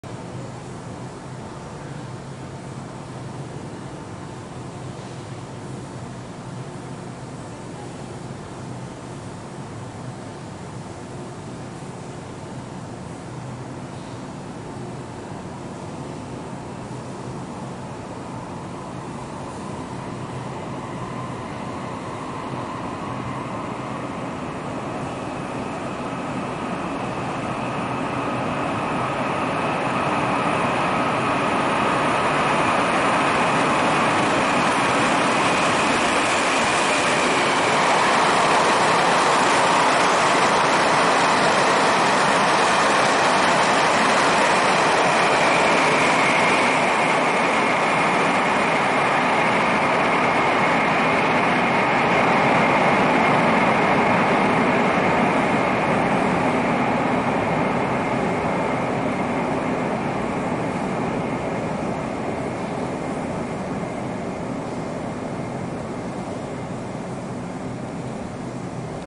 2019-06-13 左營(舊城)車站125次推拉式自強號第1月台通過
自強號  125次  經山線  七堵開往屏東